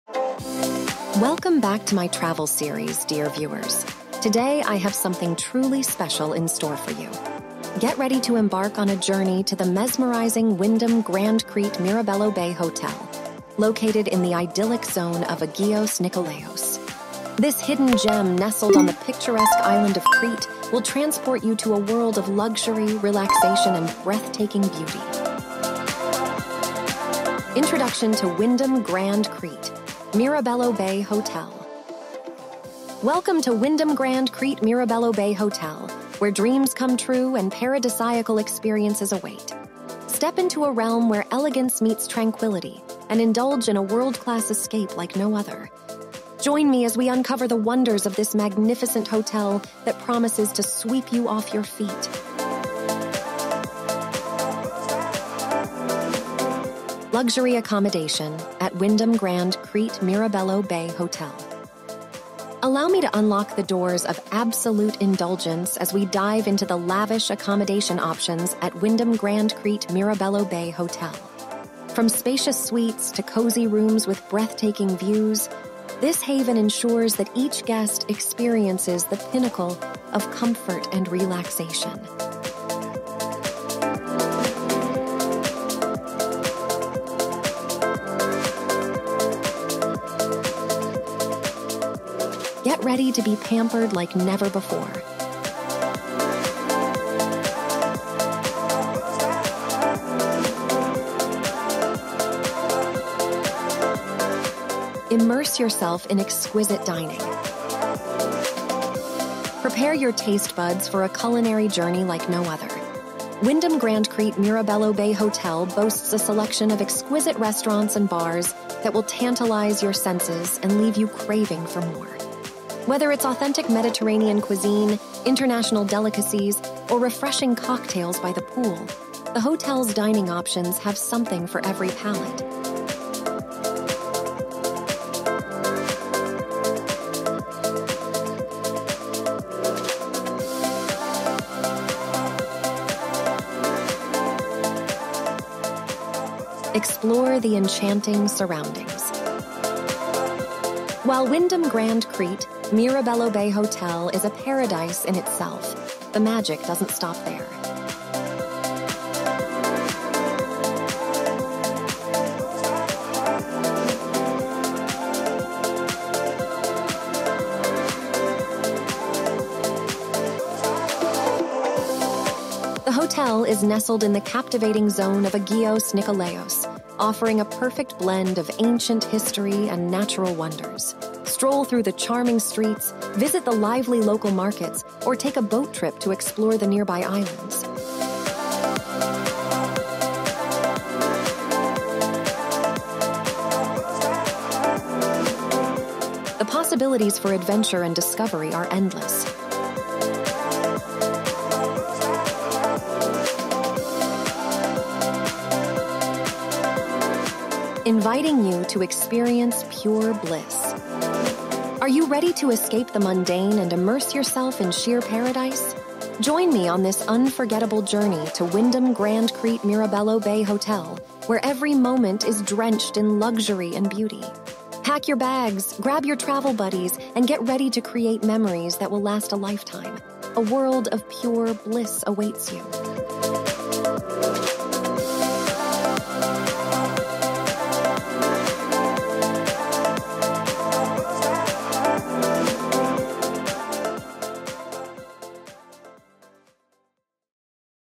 Wyndham Grand Crete Mirabello Bay Hotel 5* | Greece | Drone video
Escape to a world of luxury and tranquility at Wyndham Grand Crete Mirabello Bay Hotel! Nestled in the beautiful zone of Agios Nikolaos, this hidden gem offers breathtaking views, lavish accommodations, exquisite dining, and rejuvenating wellness facilities. Join us on this mesmerizing journey and experience the sheer paradise that awaits you. Book your stay today and indulge in the ultimate escape!

𝗩𝗶𝗱𝗲𝗼 𝗣𝗿𝗲𝘀𝗲𝗻𝘁𝗮𝘁𝗶𝗼𝗻 𝗦𝘂𝗺𝗮𝗿:
00:00 Welcome back to my travel series
00:29 Introduction to Wyndham Grand Crete Mirabello Bay Hotel
00:58 Luxury Accommodation
01:45 Immerse Yourself in Exquisite Dining
02:27 Explore the Enchanting Surroundings
03:29 Inviting You to Experience Pure Bliss